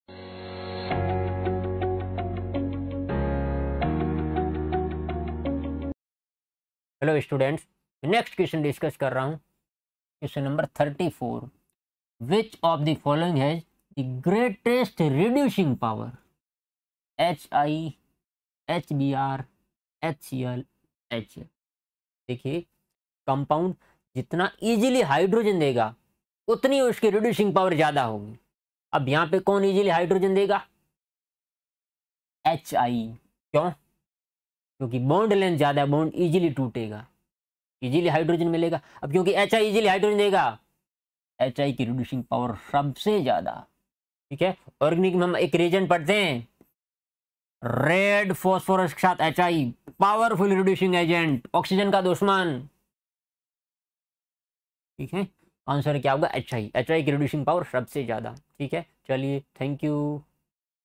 CBSE Board Paper (Basic) Term-1 | chemistry -12 | Question 34
In this video you will learn the order of reducing power for  different halogen acids.